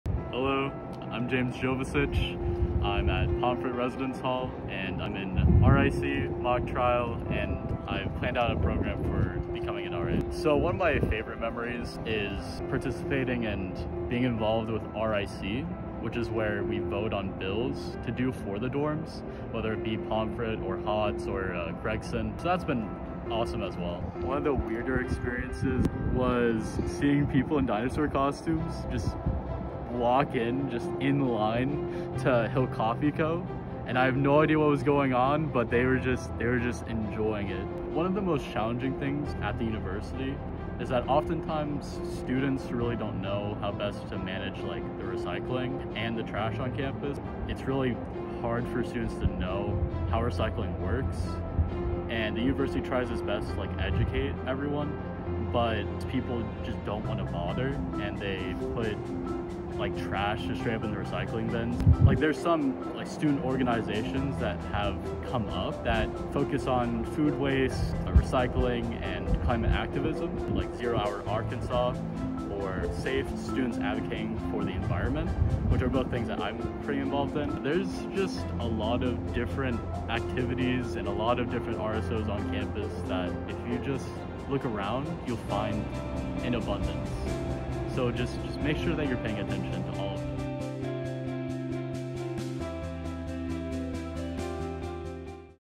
Stories With Students: James Jovicich
I had the pleasure of meeting with James Jovicich and talking to him about some of his favorite memories from living on campus. It was great to meet you James!